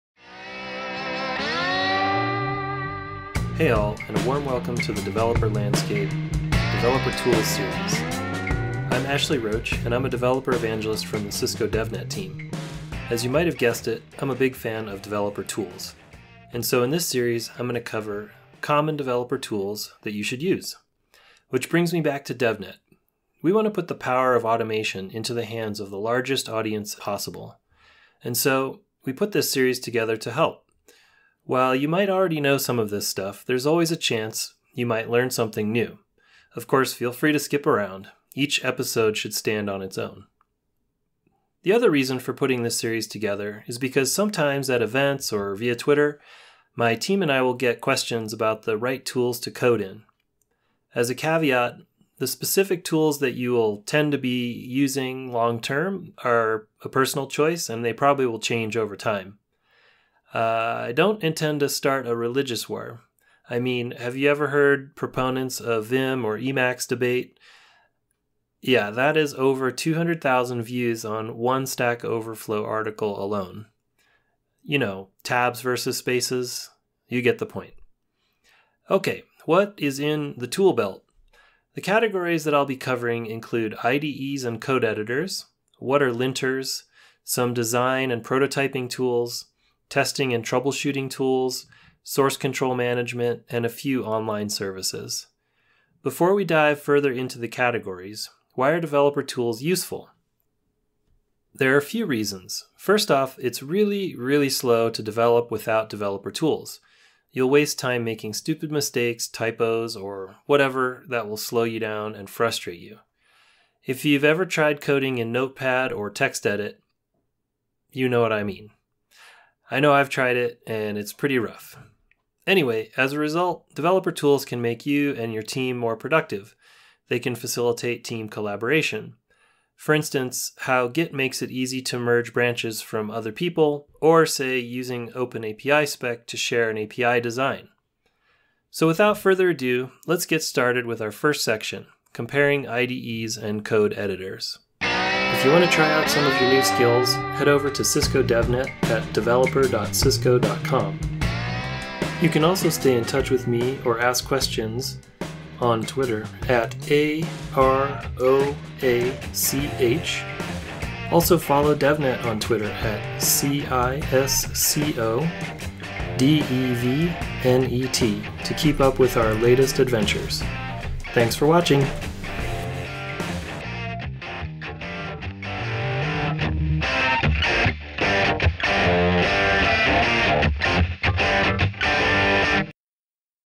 #0: Introduction to Developer Landscape: Developer Tools Series
This is the introduction to the Developer Tools Series.  I provide a high level overview of the goals of this series and what we will cover over the coming episodes.  Hosted by Ashley Roach (@aroach), Principal Engineer Evangelist for Cisco DevNet.